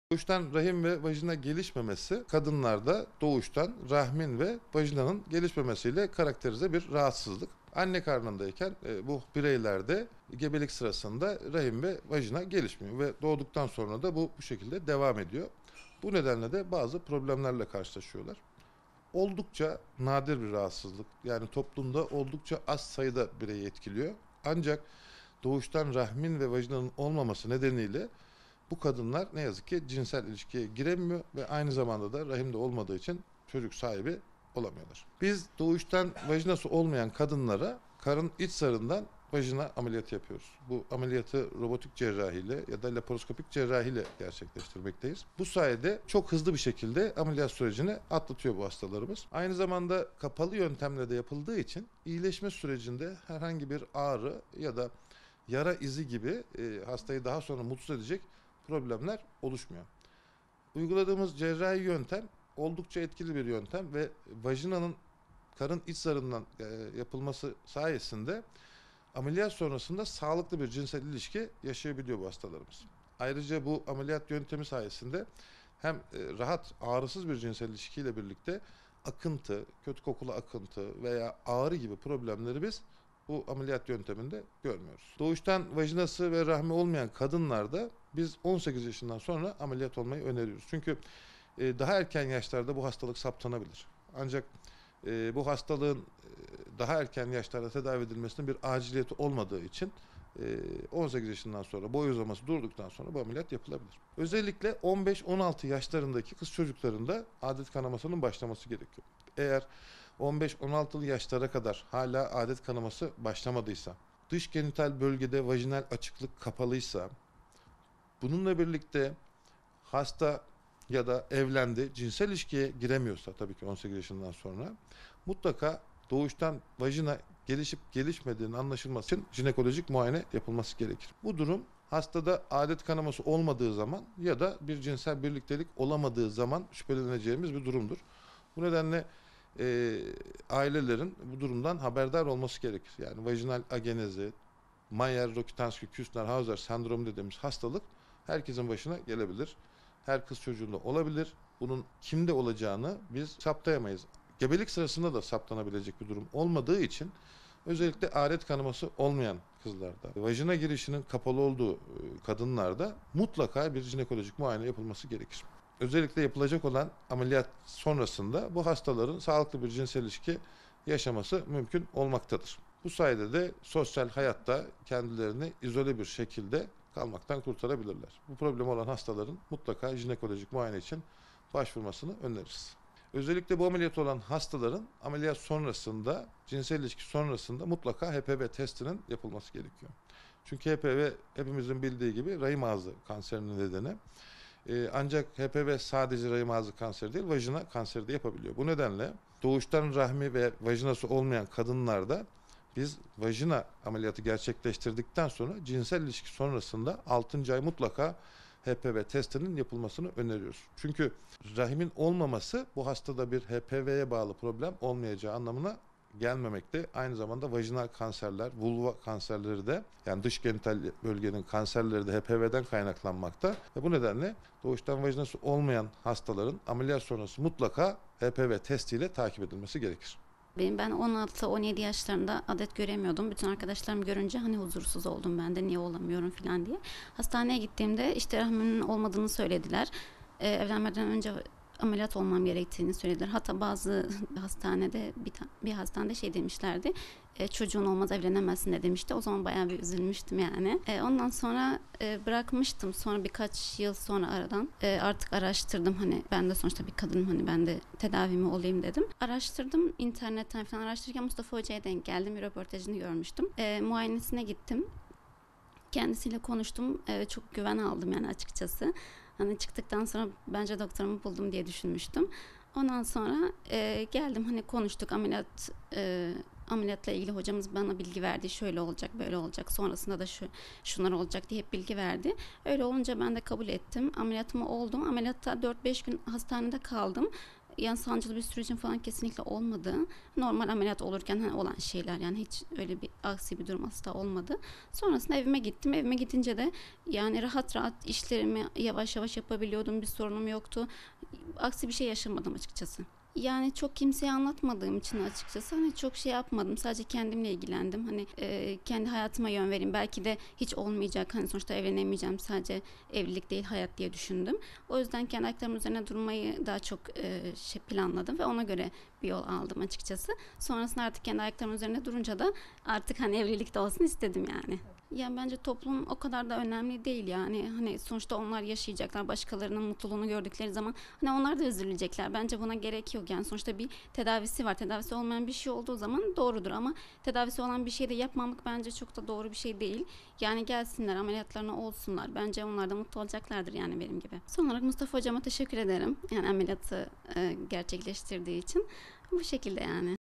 28 yaşındaki kadına, karın iç zarından bir parça alınarak vajina yapıldı
Ankara'da Kadın Hastalıkları ve Doğum Uzmanı Prof. Dr. Mustafa Ulubay'ın gerçekleştirdiği cerrahi müdahale ile doğuştan rahmi ve vajinası olmayan G.G.'ye (28) karın iç zarından alınan parça ile vajina oluşturuldu. Başarılı geçen ameliyatın ardından sağlığına kavuşan G.G., "Sancılı bir sürecim kesinlikle olmadı. Sonrasında evime gittim, işlerimi yavaş yavaş yapabiliyordum" dedi.